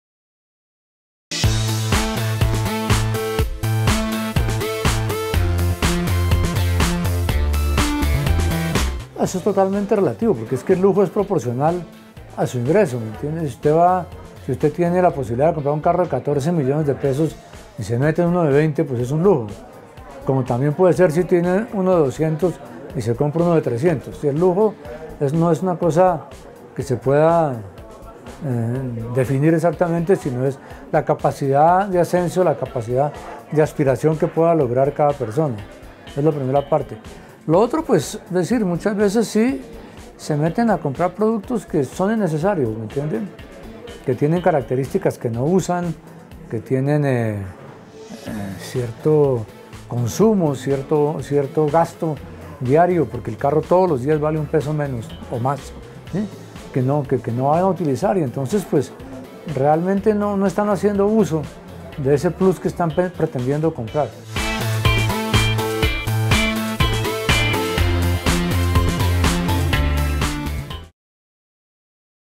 ¿Comprar carro nuevo es un lujo o una necesidad?
José Clopatofsky, director de la Revista Motor,  nos explica como hacer una buena inversión a la hora de adquirir un vehículo.